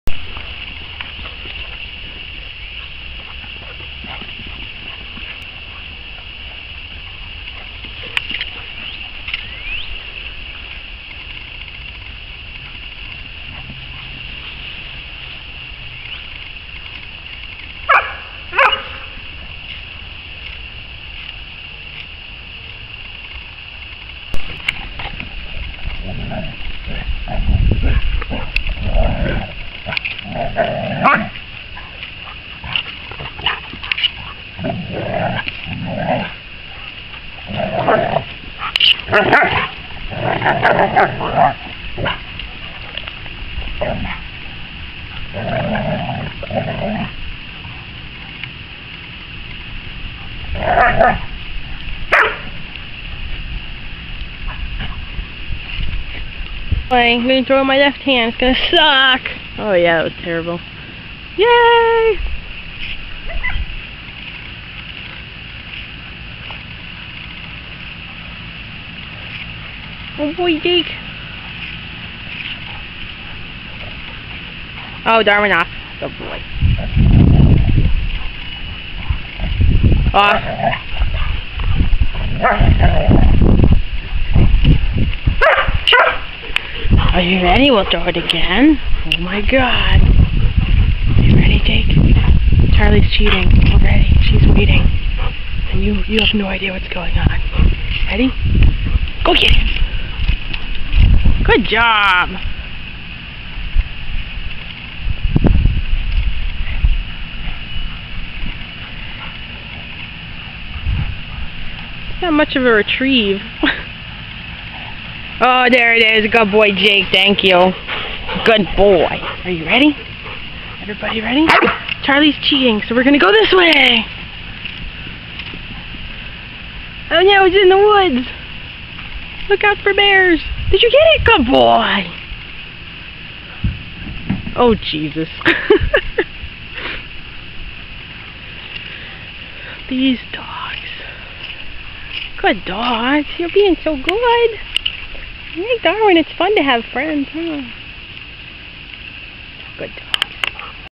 Darwin's New Friends
I'm petsitting for my old boss's 11 month old collie puppy so I'm uploading this for her so she can see how much fun Darwin is having! This is him playing with Charlie at my house and then Charlie and Jake at my parents' house. They are teaching him their retriever ways!